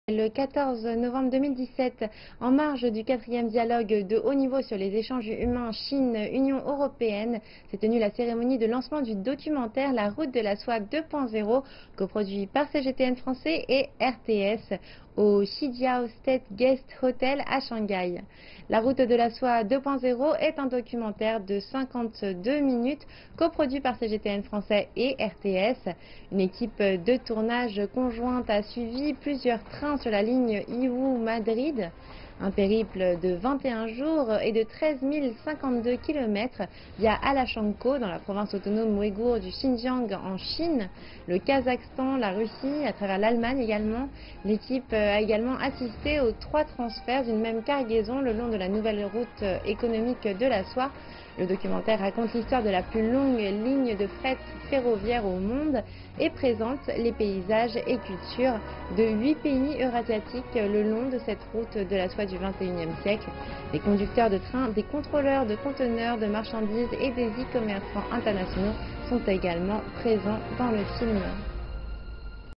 Cérémonie de lancement du documentaire sino-suisse à Shanghai
Le 14 novembre 2017, en marge du 4ème Dialogue de haut niveau sur les échanges humains Chine-UE, s'est tenue la cérémonie de lancement du documentaire La Route de la soie 2.0, coproduit par CGTN-Français et RTS, au Xijiao State Guest Hotel à Shanghai. La Route de la soie 2.0 est un documentaire de 52 minutes coproduit par CGTN-Français et RTS. Une équipe de tournage conjointe a suivi plusieurs trains sur la ligne Yiwu-Madrid. Un périple de 21 jours et de 13 052 km via Alashankou dans la province autonome ouïghoure du Xinjiang en Chine, le Kazakhstan, la Russie et l'Allemagne. L'équipe a également assisté aux 3 transferts d'une même cargaison le long de la Nouvelle Route économique de la soie. Le documentaire raconte l'histoire de la plus longue ligne de fret ferroviaire au monde et présente les paysages et cultures de 8 pays eurasiatiques le long de la Route de la Soie du XXIème siècle. Des conducteurs de train, des contrôleurs de conteneurs de marchandises et des e-commerçants internationaux sont également présents dans ce film.